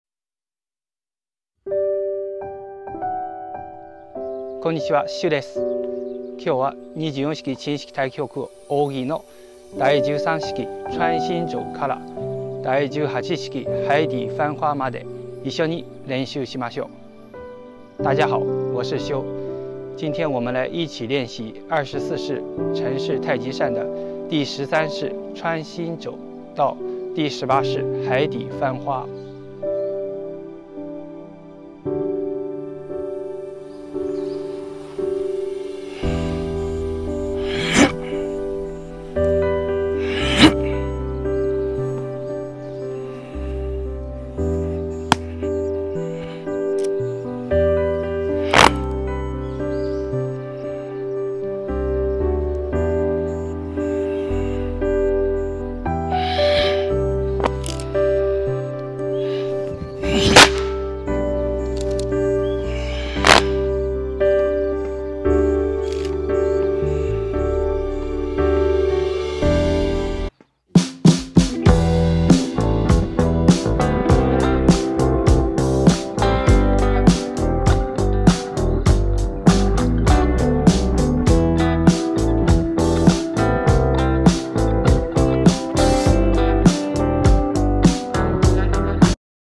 24式陳式太極扇 第十三式穿心肘〜第十八式海底翻花 24shi Chenshi taiji shan | 太極修練TaijiShuren
24式陳式太極扇 第十三式穿心肘〜第十八式海底翻花
一緒に練習しましょう
一起練一遍
Let’s practice together

⬇️【24式陳式太極扇第十三式穿心肘〜第十八式海底翻花背面動画】⬇️